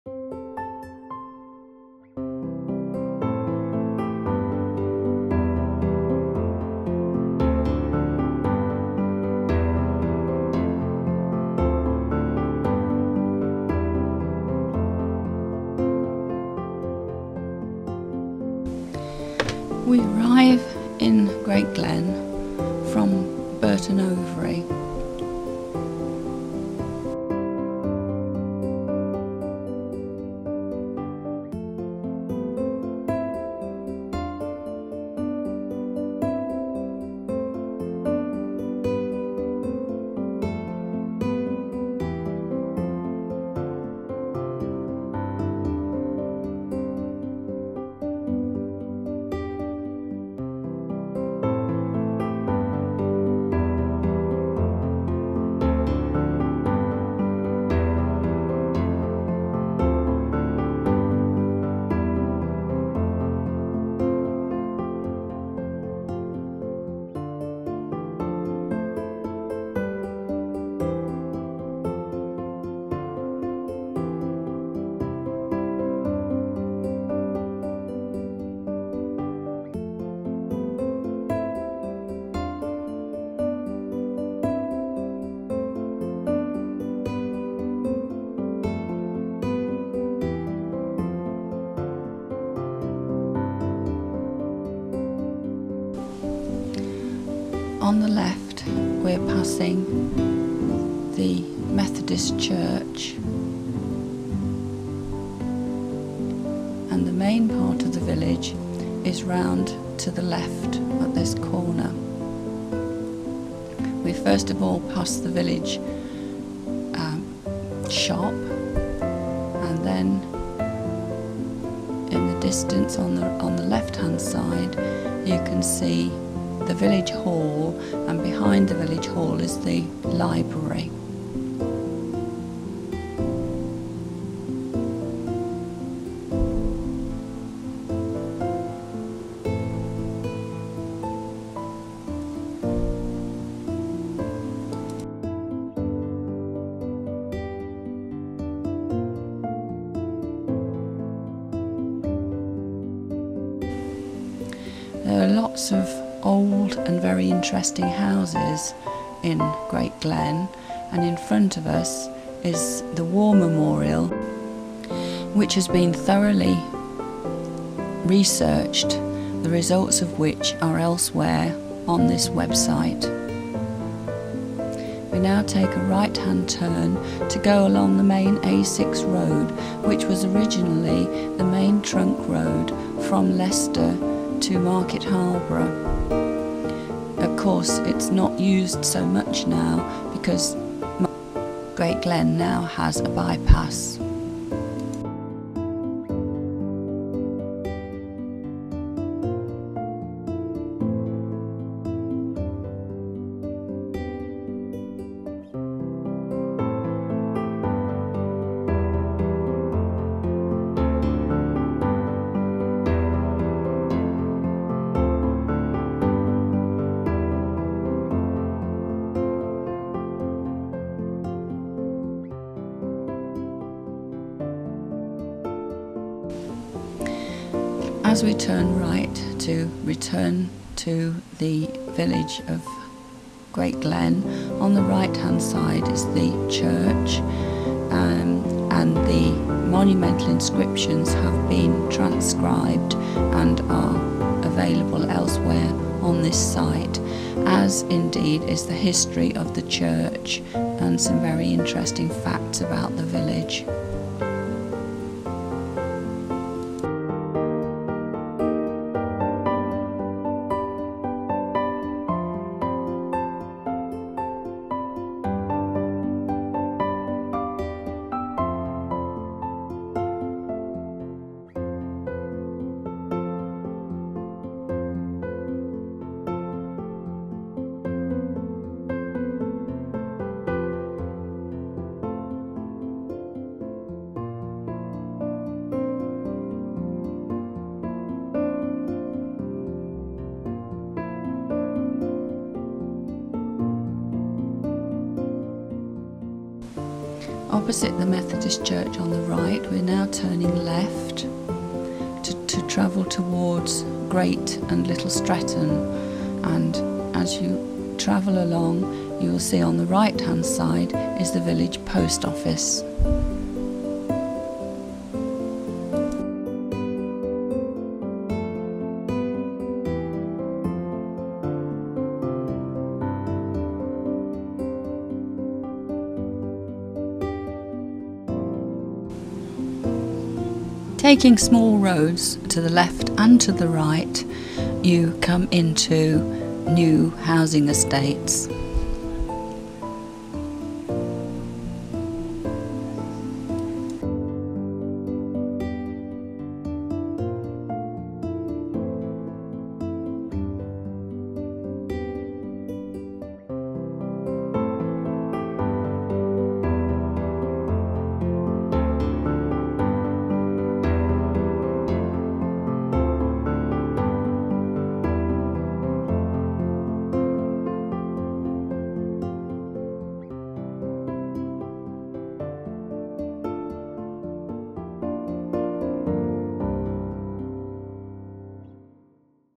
A drive through the Leicestershire village of Great Glen
Taken for the Leicestershirevillages web site